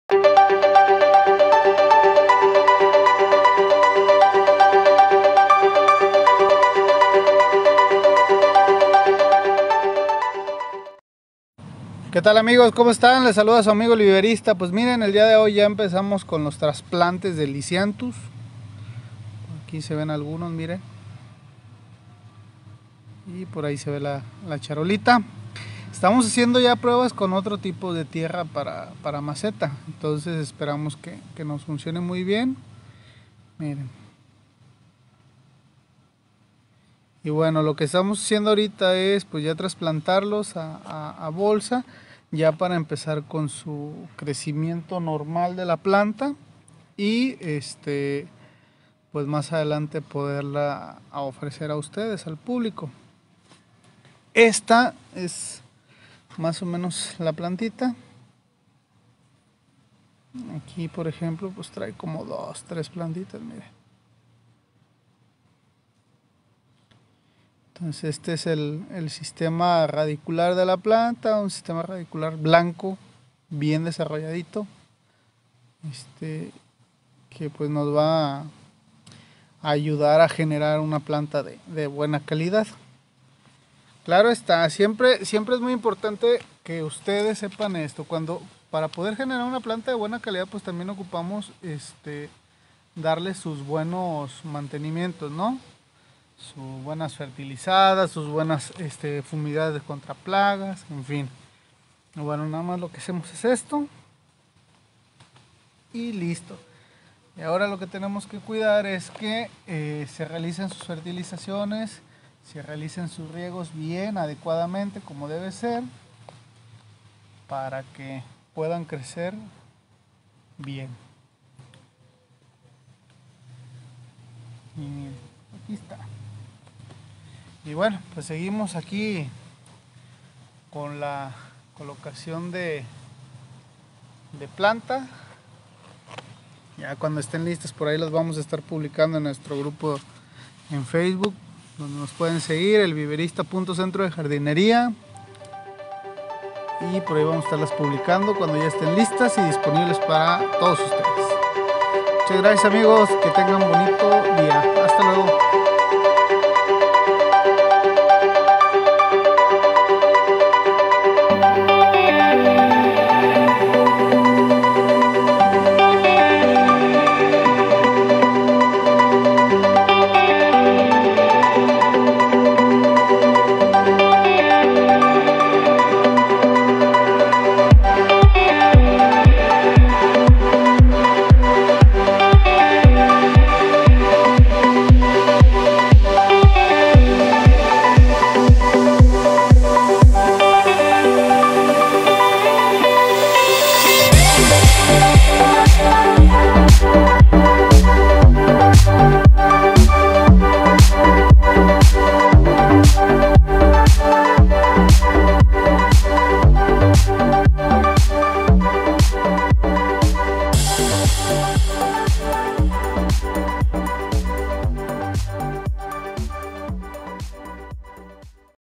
"APRENDE" trasplantando lisianthus con El viverista
VEN Y DESCUBRE LO QUE SE HACE AL MOMENTO DEL TRASPLANTE DEL LISIANTHUS